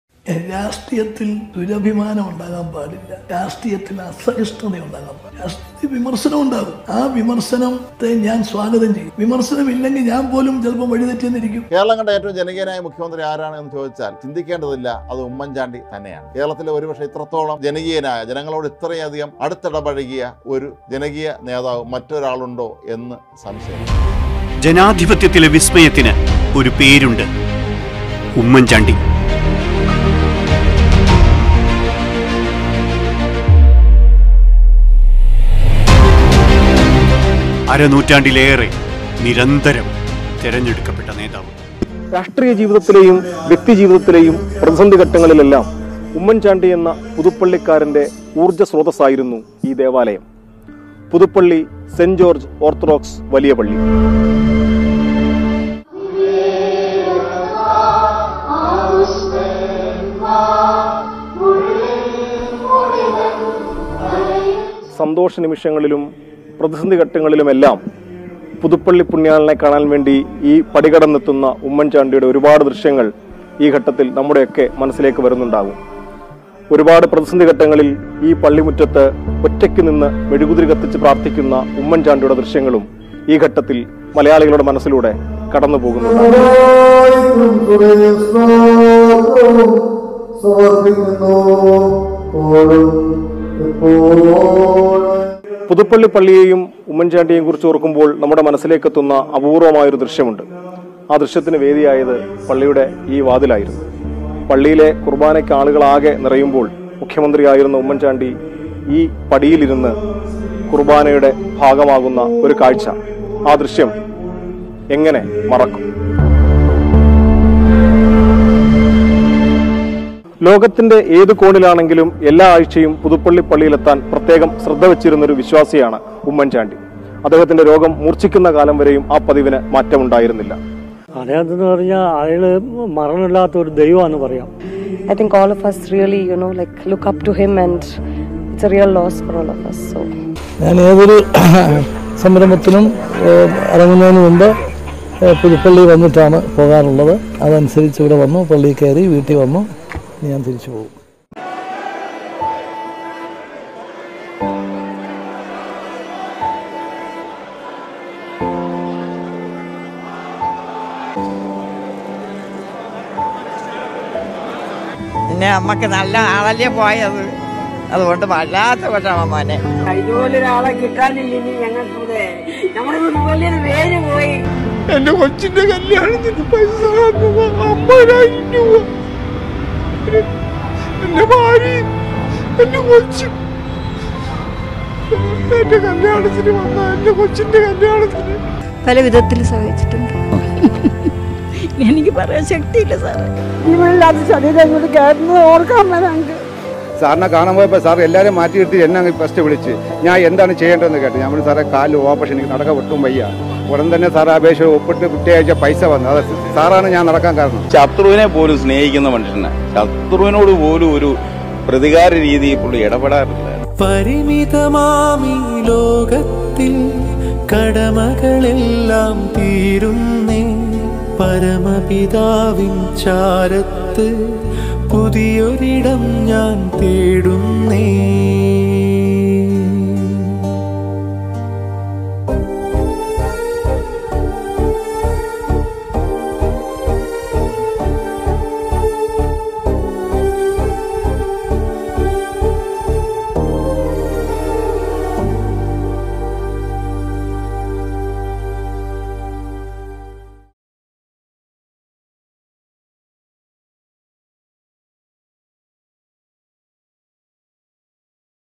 ഉമ്മൻ ചാണ്ടി എന്ന നേതാവും കുഞ്ഞൂഞ്ഞ് എന്ന ദൈവ വിശ്വാസിയും.
In the rememberence of former chief minister oomman chandy